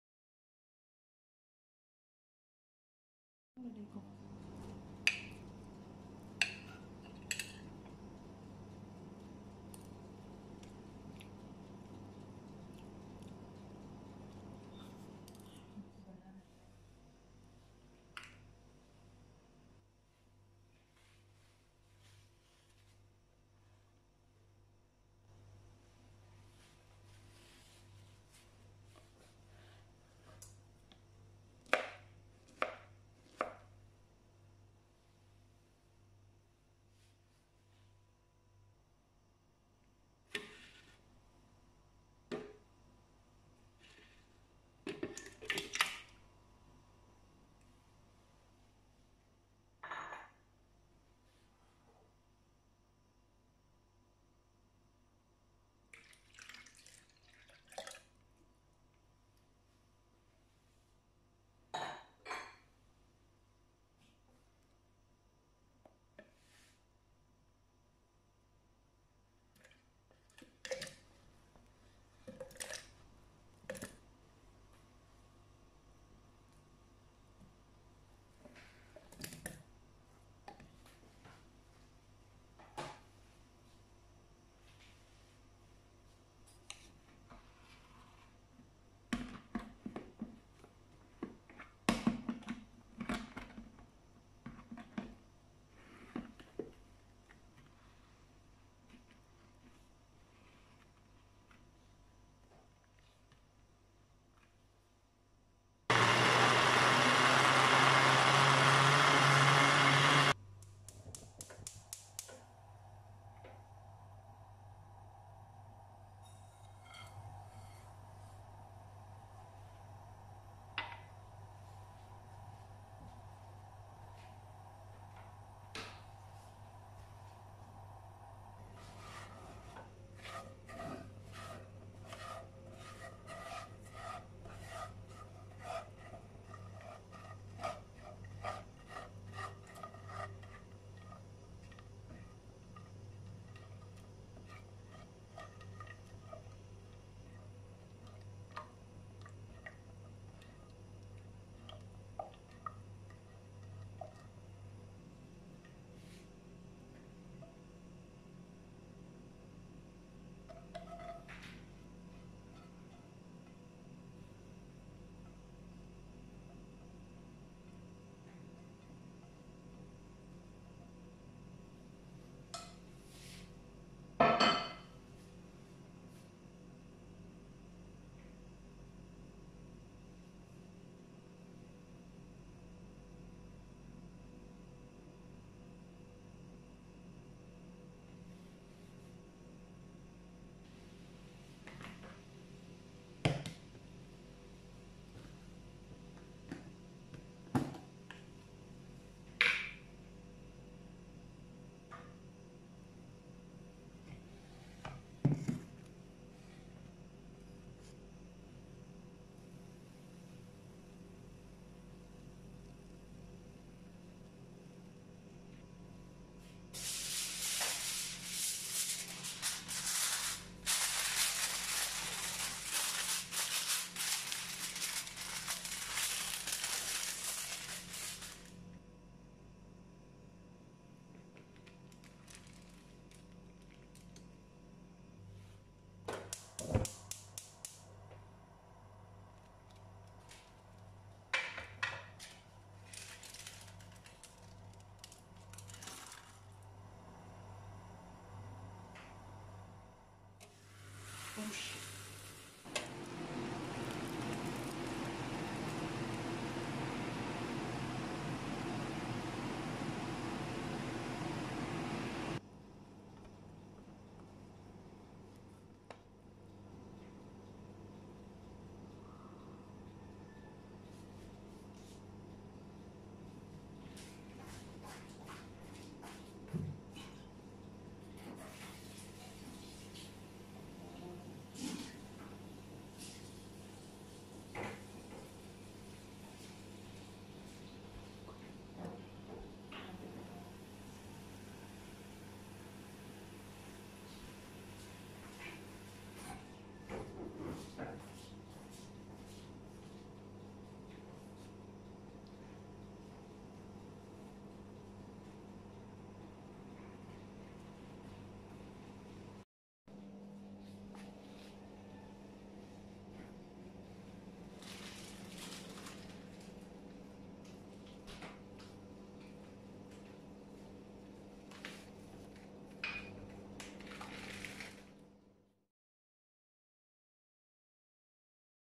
banana cake without flour|flan cake|Gioia
This recipe is very quick and easy in just 3 ingredients you can have delicious dessert or snack anytime .try this at home

All you need:

4 eggs
4 bananas
400 ml milk

@sanjuanjoy@gmail. com